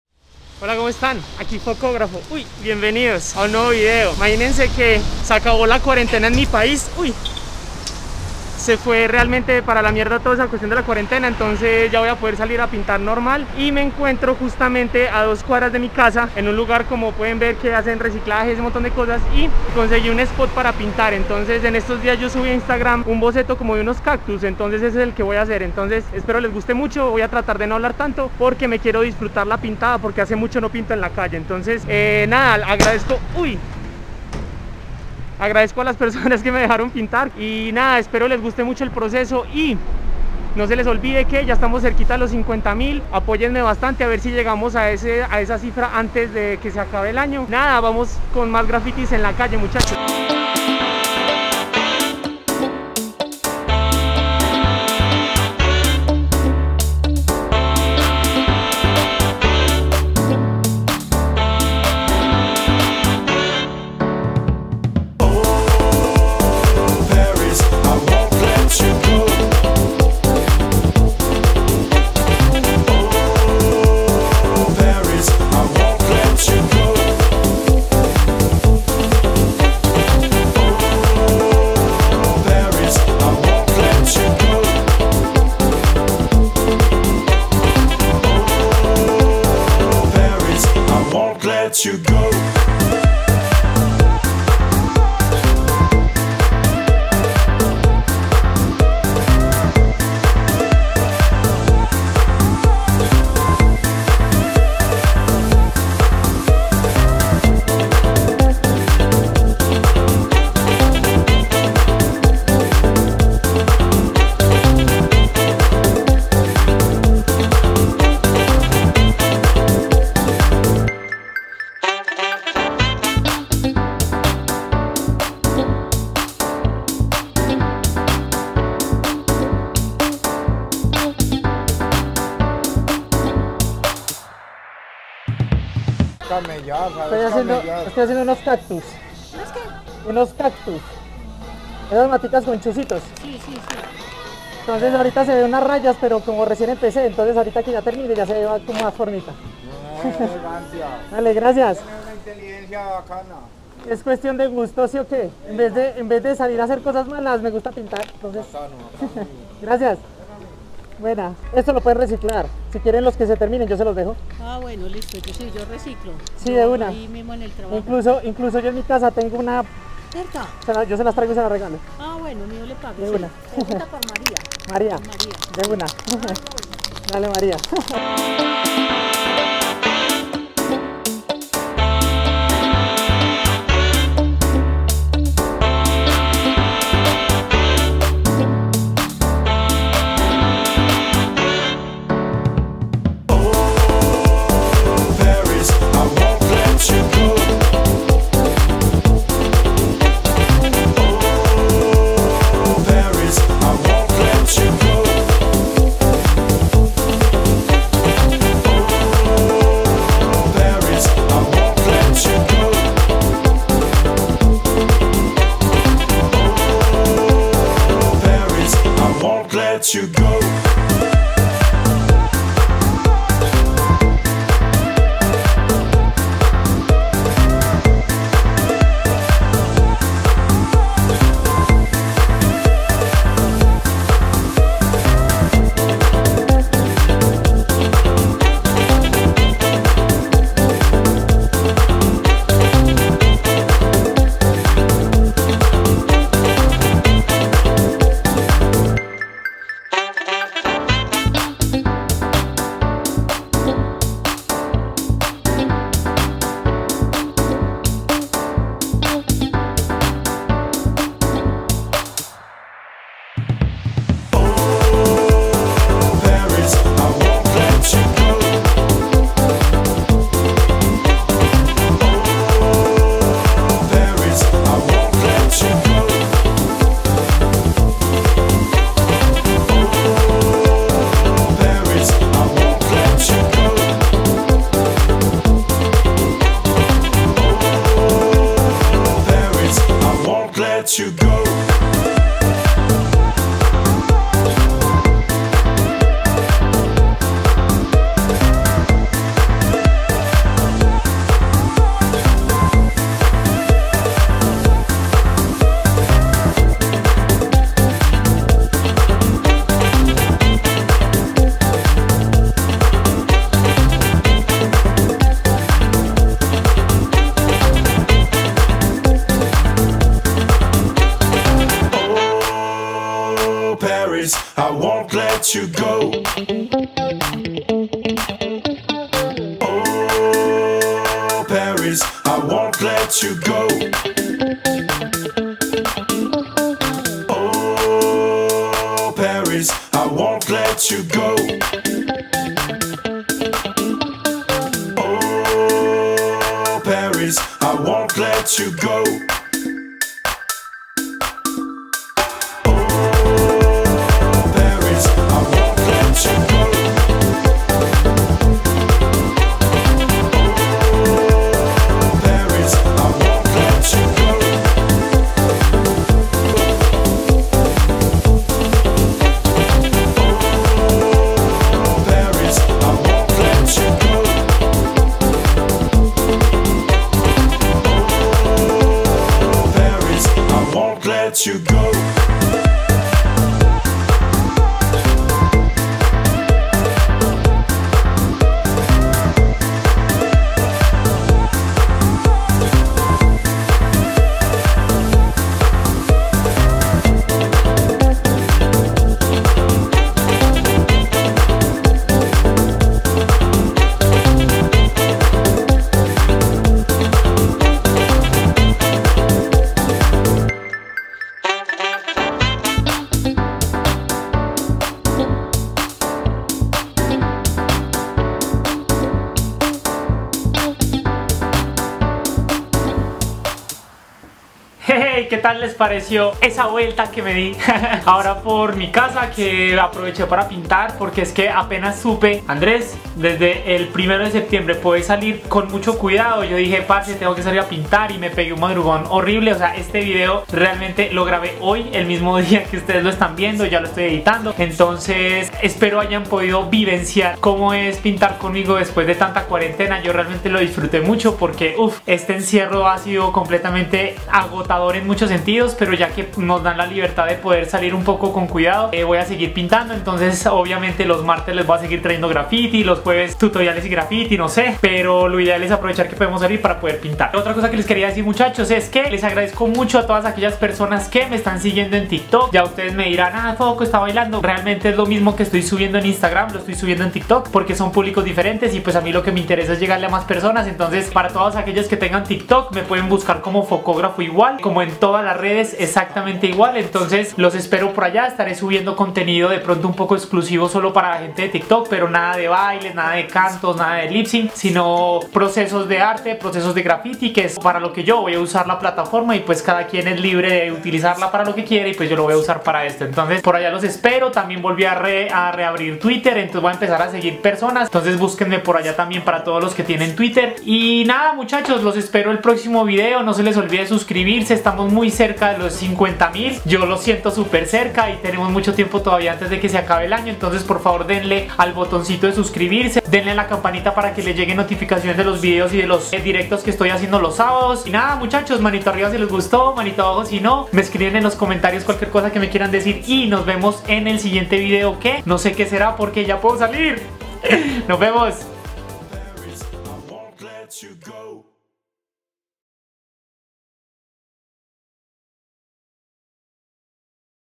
Salgo por fin a pintar a la calle, y que mejor que con una salsita bien bailable
Si van a salir, con cautela llaves.

Música